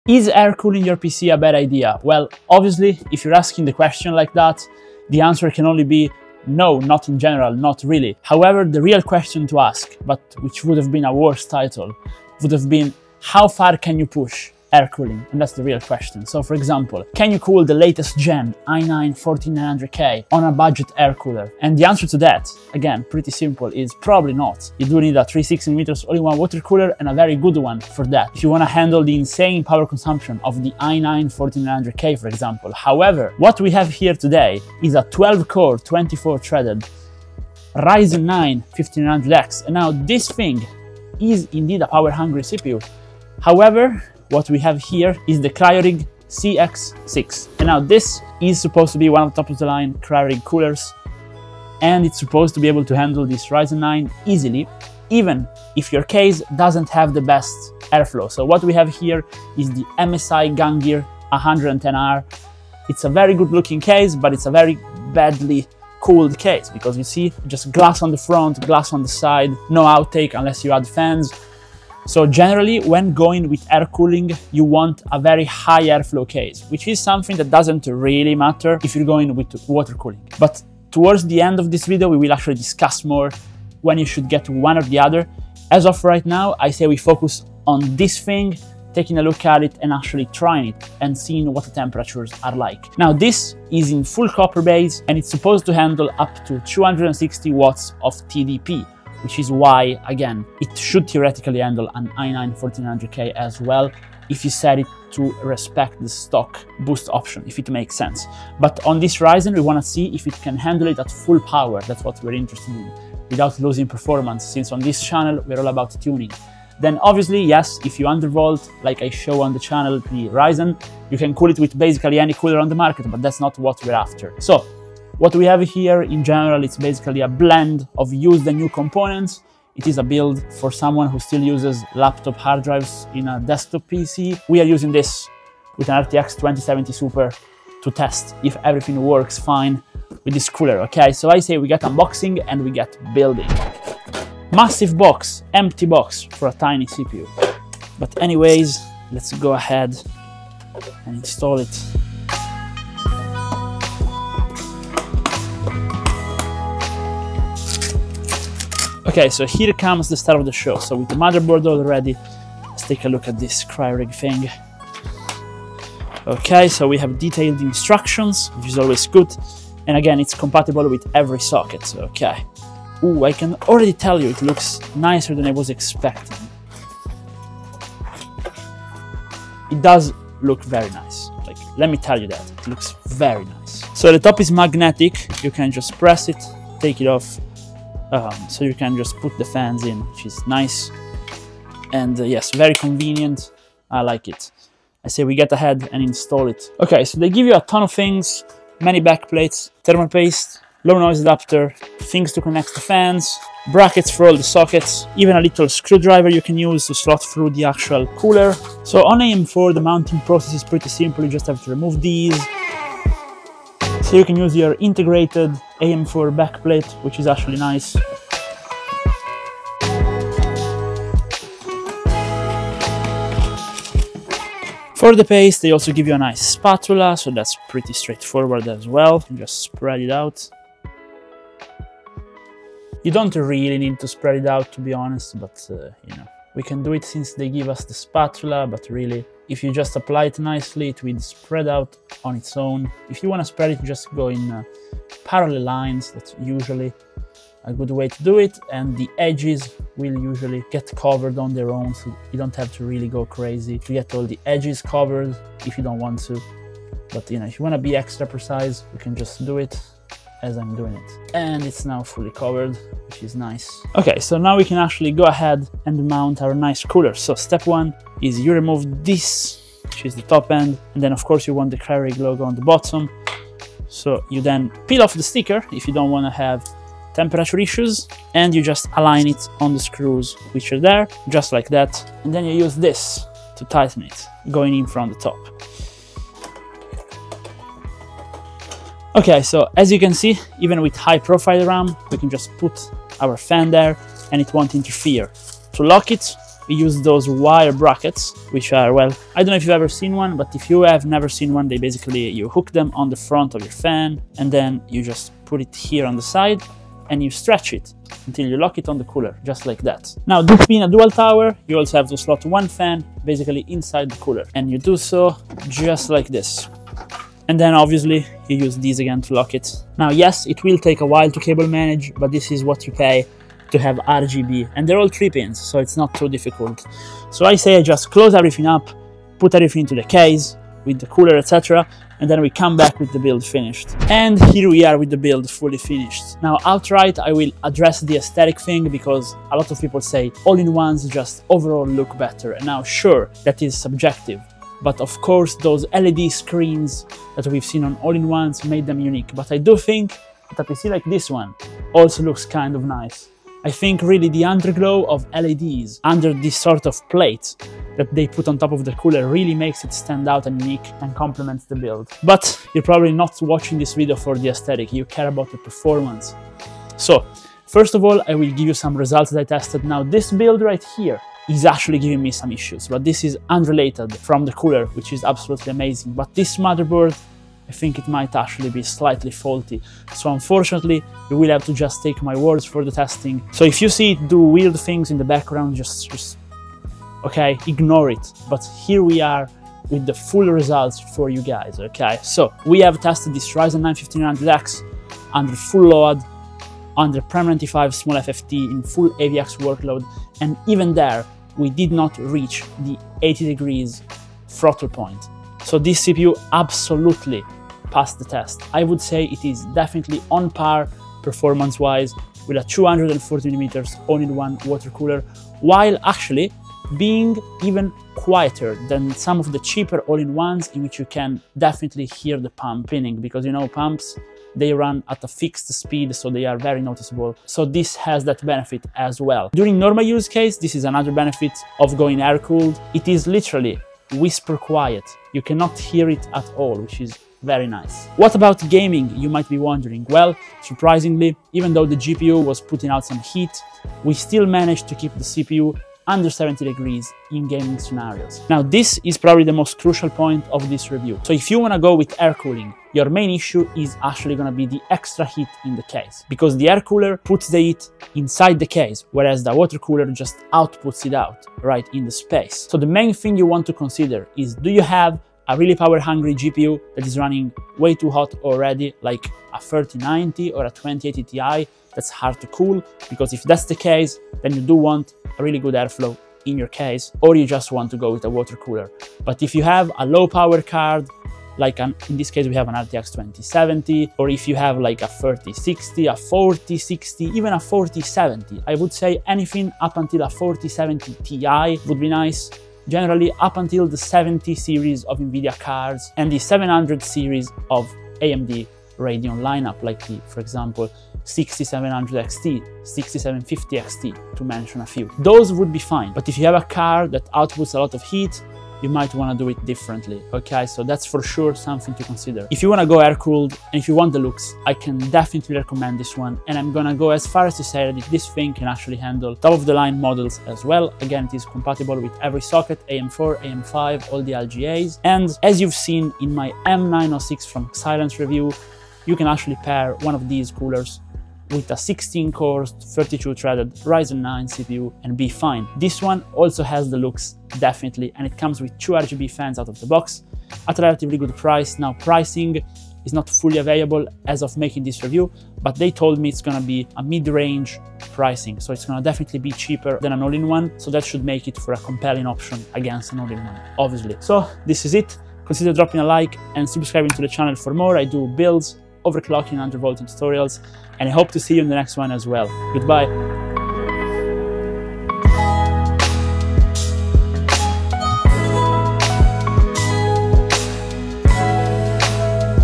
Should you still buy an Air Cooler? | Cryorig CX6 Review
Nowadays 90% of PCs that mount an aftermarket cooler are watercooled and rightfully so: watercooling is cheap, easy to install, works well and perform nicely.

...however, there are still some air coolers out there worth buying! Among Noctua, Xilence and DeepCool, Cryorig definitely competes, wiith the new CX-6